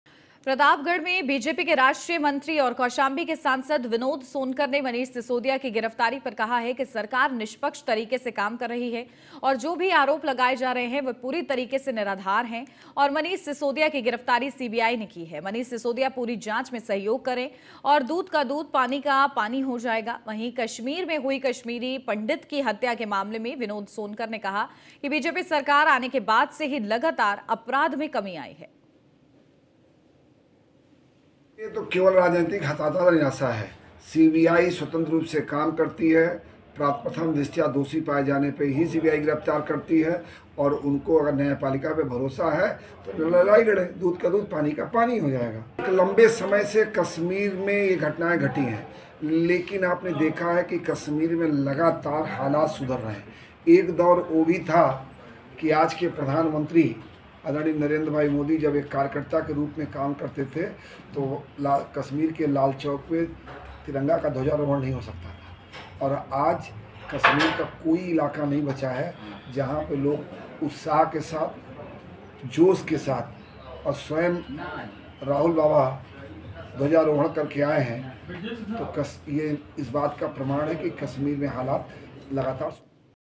Pratapgarh: बीजेपी के मंत्री Vinod Sonkar ने Manish Sisodia की गिरफ्तारी पर कही बड़ी बात
Airtel - 350,
Netvision - 219,
RCN - 403,
UCN-223
Alisha Digital- 285
HDS- 127

We are also available on:
MX Player
JIO TV